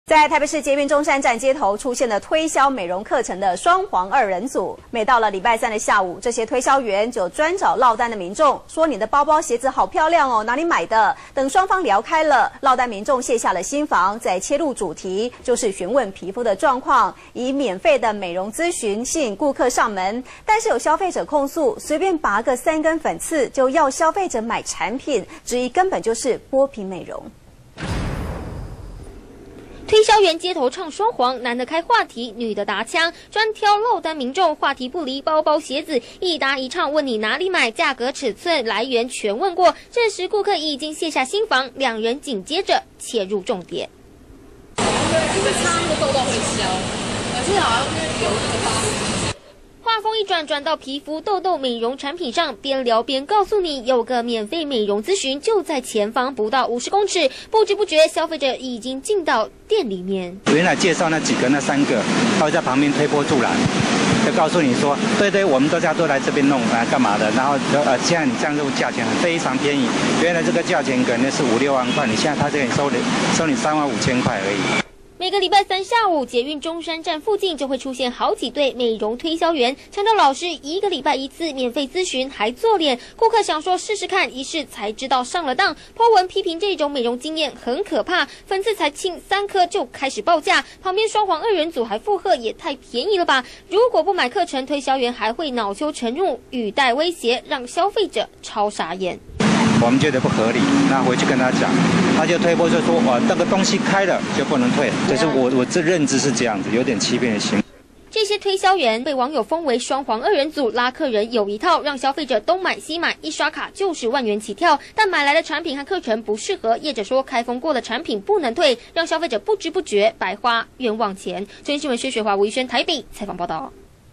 直擊！推銷唱雙簧 先聊鞋包再推銷護膚
中天新聞》台北市捷運中山站街頭近日出現推銷美容課程的「雙簧二人組」，每周三下午這些推銷員都專挑落單民眾，以包包、鞋子等服飾配件開話題，等對方卸下心防再切入主題詢問皮膚狀況，以免費美容諮詢吸引顧客上門，不過有消費者向媒體控訴，隨便拔3根粉刺，就開價要消費者買產品，質疑根本是剝皮美容。